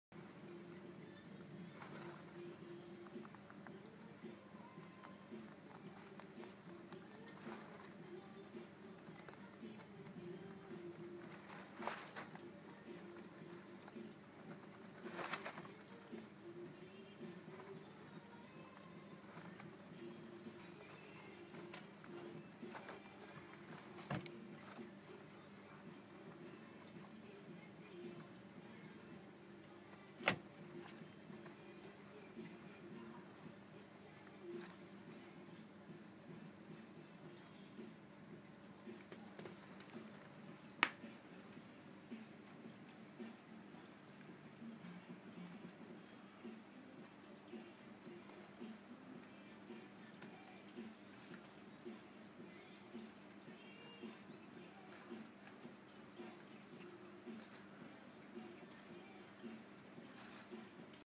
ロップイヤー(ホーランドロップ)の赤ちゃん⑪
ロップイヤー(ホーランドロップ)の赤ちゃんです。生後１４日目です。お母さんと一緒。